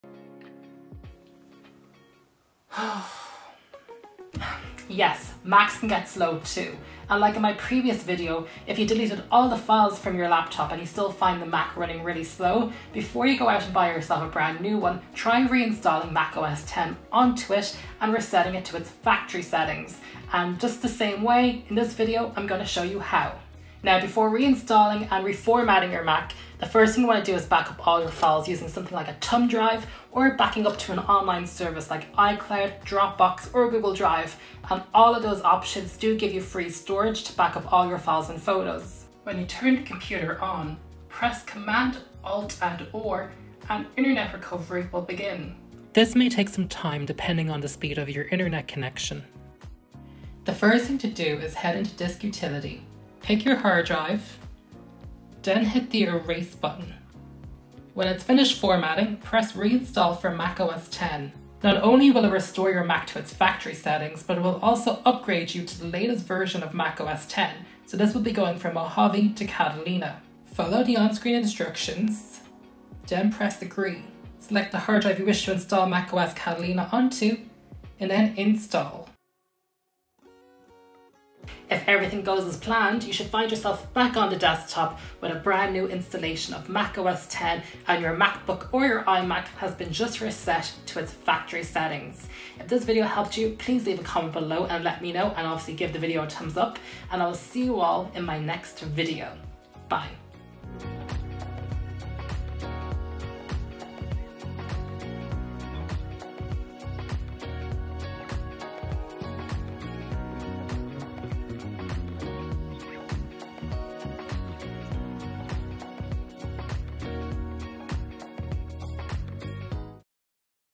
Easily RESET your Mac to its Factory Settings
In this video I show you how to really easily re-install MacOS on your MacBook and iMac - the video's just 2 minutes long! This video covers how to easily reset recent Macs to their factory settings before you think to go out and buy a new one.

I also recorded this video on my "new" Sony camera which I'm super excited about - I finally graduated from the iPhone camera!

THIS VIDEO REVIEW IS INDEPENDENT AND DOES NOT CONTAIN ANY SPONSORSHIP OR PRODUCT PLACEMENT.